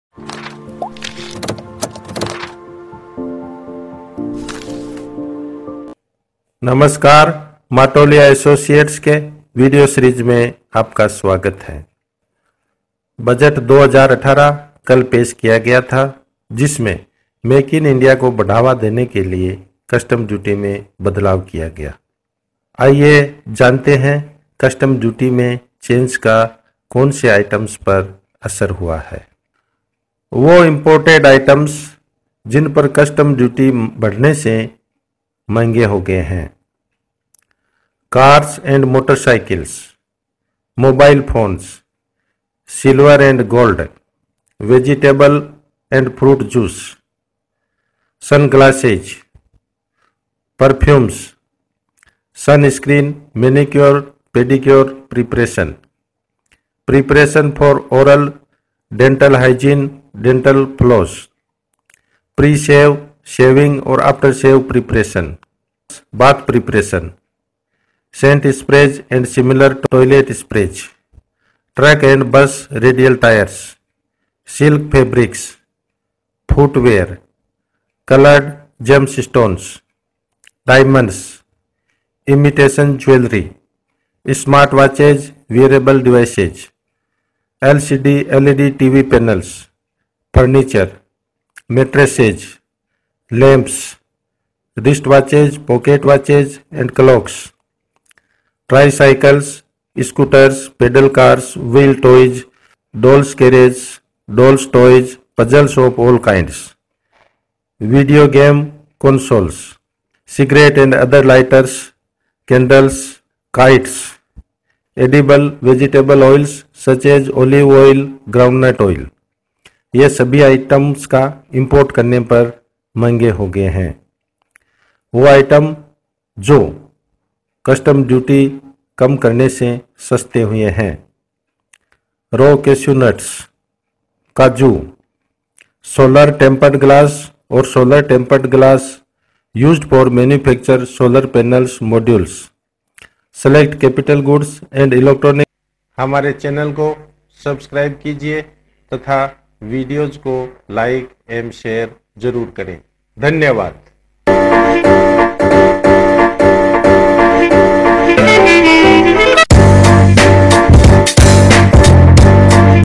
Part 2- Union Budget 2018 क्या हुआ सस्ता महंगा | List of items that became Costlier or Cheaper
जानिए बजट 2018 में क्या हुआ सस्ता या महंगा |

The following is a list of imported items that will become costlier as increase in Custom Duty
• Cars and motorcycles
• Mobile phones
• Silver
• Gold
• Vegetable, fruit juices
• Sunglasses
• Perfumes
• Sunscreen, manicure, pedicure preparations
• Preparations for oral dental hygiene, dental floss
• Pre-shave, shaving or after-shave preparations,
• Deod orants, bath preparations
• Scent sprays and similar toilet sprays
• Truck and Bus radial tyres
• Silk Fabrics
• Footwear
• Coloured gemstones
• Diamonds
• Imitation jewellery
• Smart watches/wearable devices
• LCD/ LED TV panels
• Furniture
• Mattresses
• Lamps
• Wrist watches, pocket watches, clocks
• Tricycles, scooters, pedal cars, wheeled toys, dolls’ carriages, dolls, toys, puzzles of all kinds
• Video game consoles
• Articles and equipment for sports or outdoor games, swimming pools and paddling pools
• Cigarette and other lighters, candles
• Kites
• Edible/vegetable oils such as olive oil, groundnut oil

The following is the list of imported items that will become cheaper as reduction in Custom Duty
• Raw cashew nuts
• Solar tempered glass or solar tempered glass used for manufacture solar panels/modules
• Select capital goods and electronics such as ball screws and linear motion guides

Other items prices reduced as result of Reduction in Excise Duty
• Petrol and diesel

MATOLIA ASSOCIATES TAX CONSULTANTS